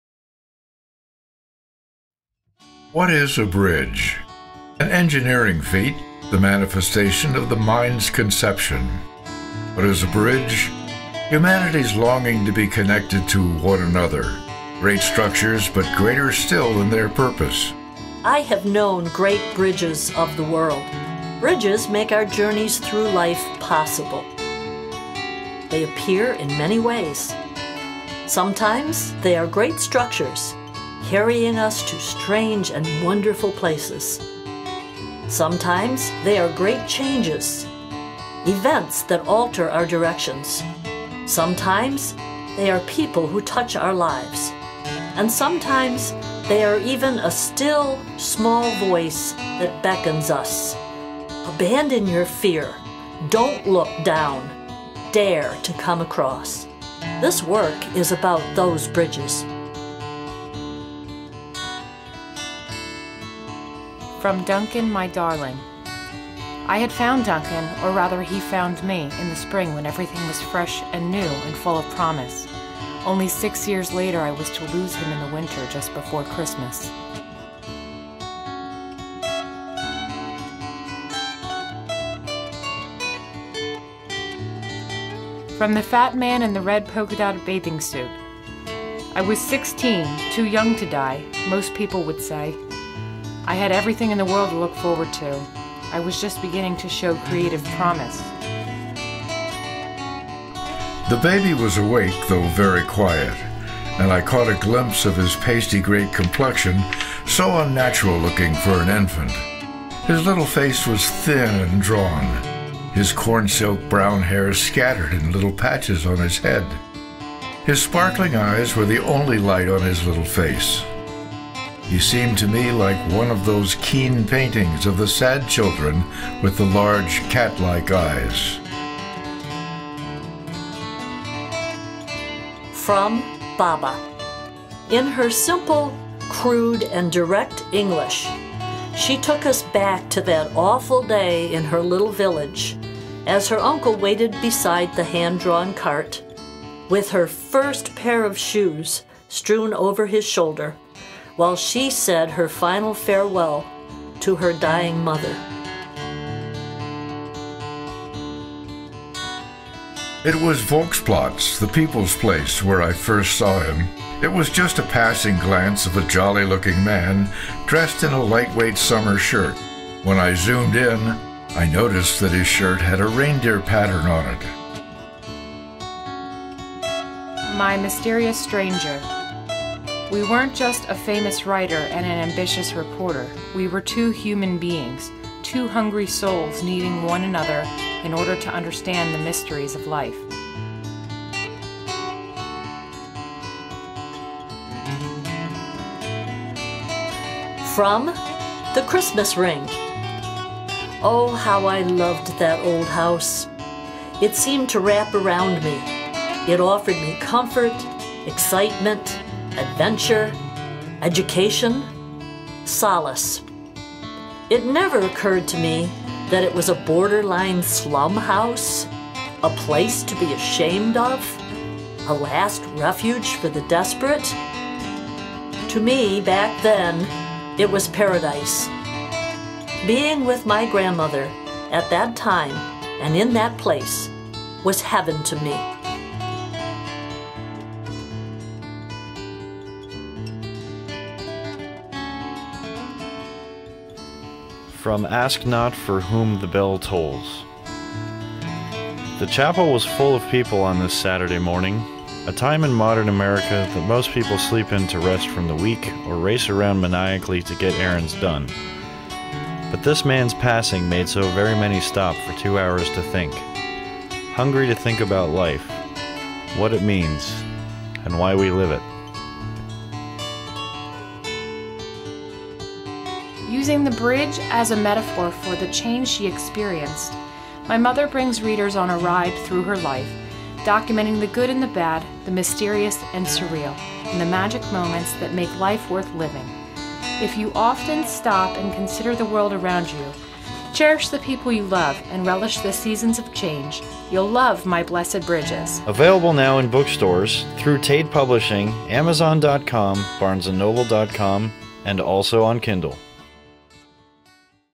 Blessed Bridges Promo Video
a Sample reading from Bobbie Trotter's book, "My Blessed Bridges" with original music by Ross Trotter